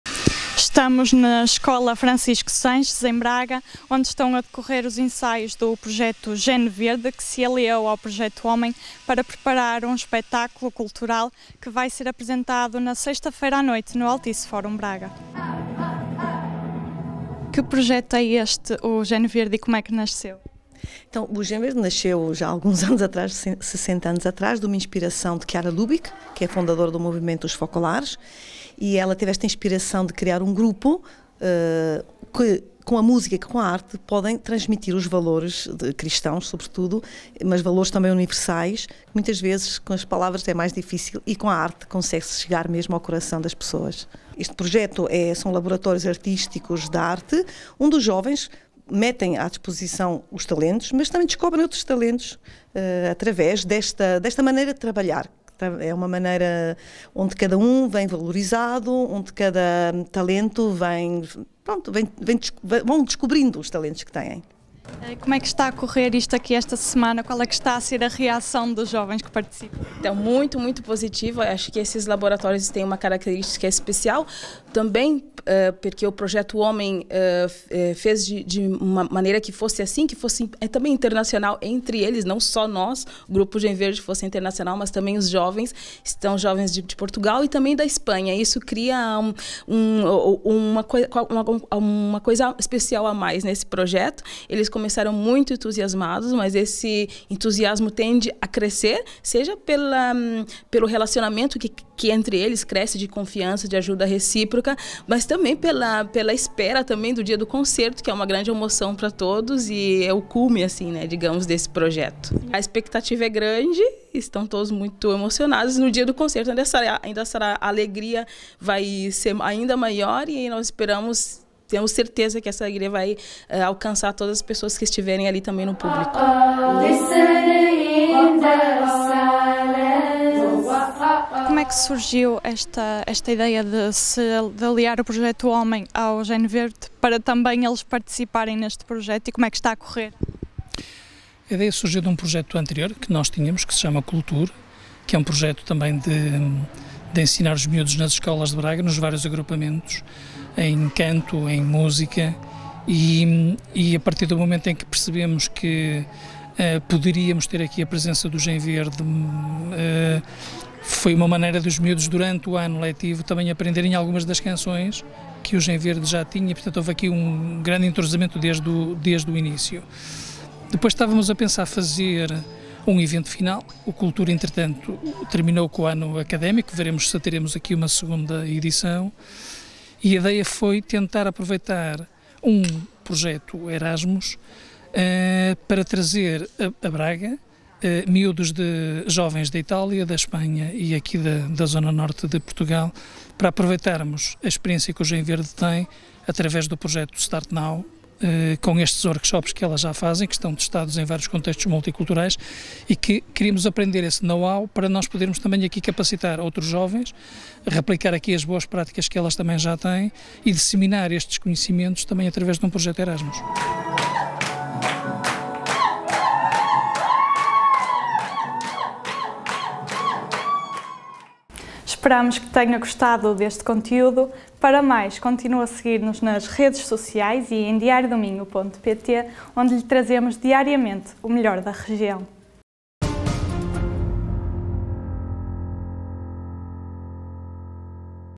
Gen Verde e Projeto Homem transmitem mensagens fortes e atuais através da arte
O grupo internacional Gen Verde – International Performing Arts Group está em Braga durante esta semana, durante a qual está a realizar um conjunto de workshops gratuitos com jovens dos Agrupamentos Escolares da cidade e do Projeto Homem, que vão culminar num espetáculo que se realiza na sexta-feira no Altice Forum Braga.